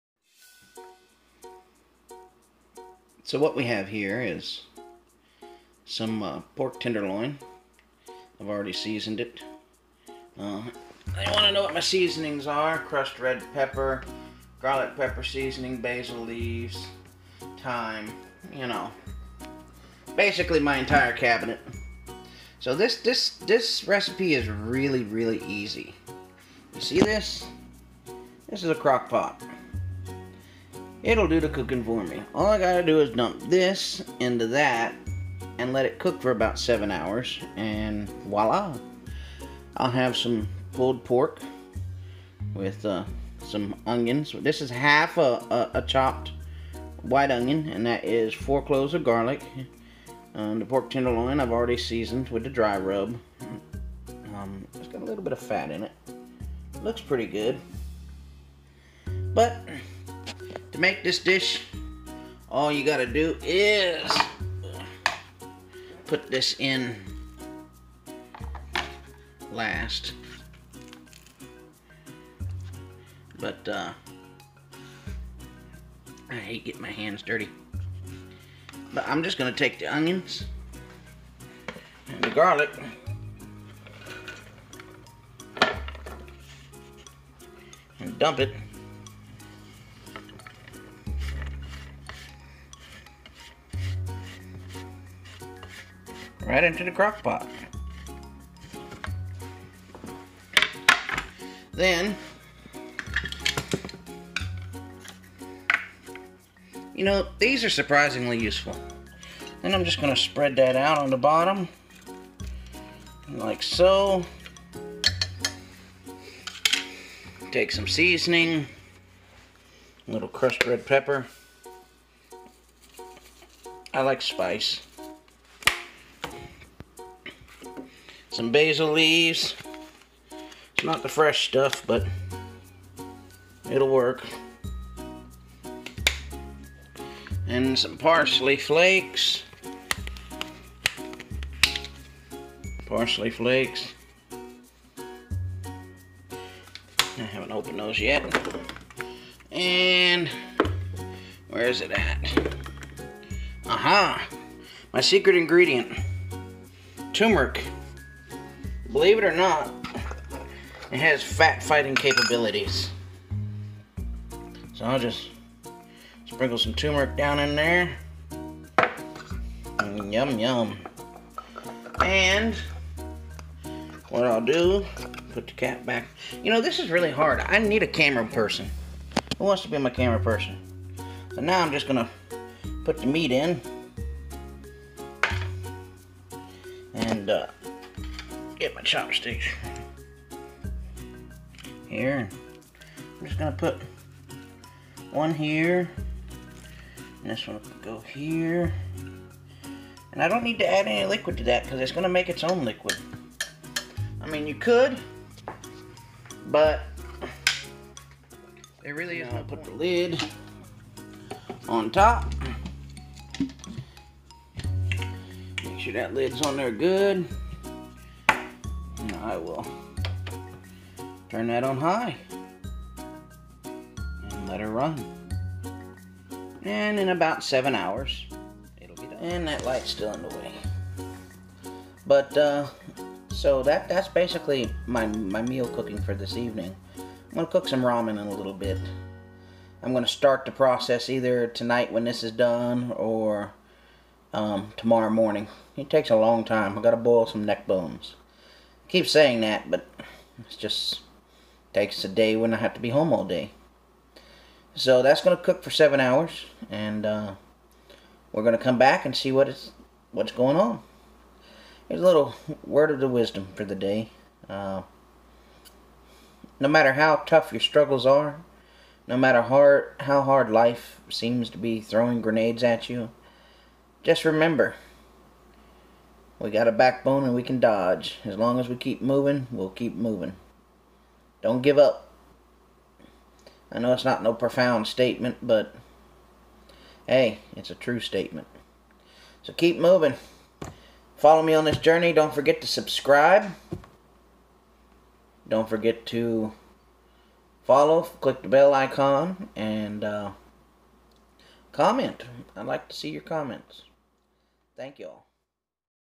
Cheap simple crock pot dinner/ HEALTHY Cooking
A simple crock pot pulled pork dinner.
Pork tenderloin
A white onion
4 cloves of garlic
Season to.taste
7 hours on high
Yum